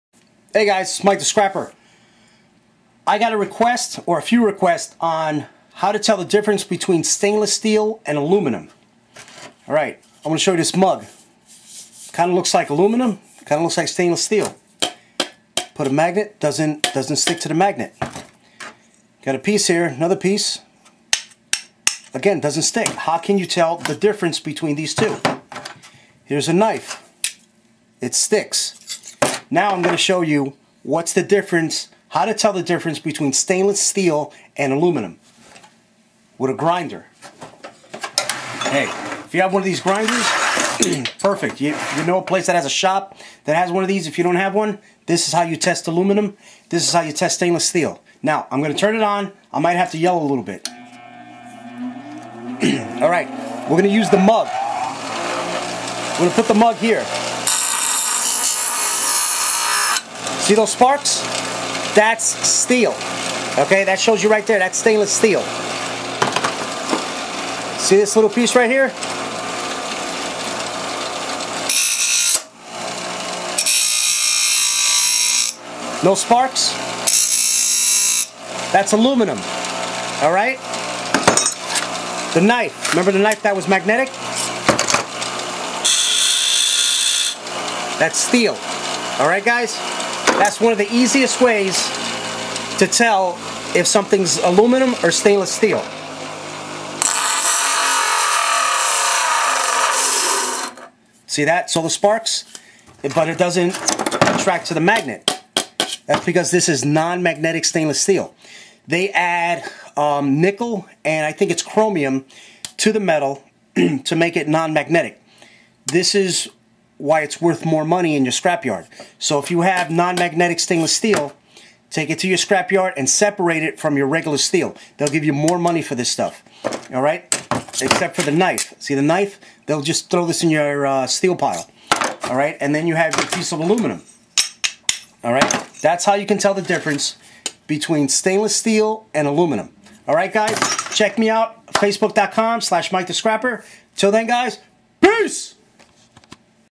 How to Recognized Stainless steel or Aluminum
How to recognizing is something build from stainless steel or aluminum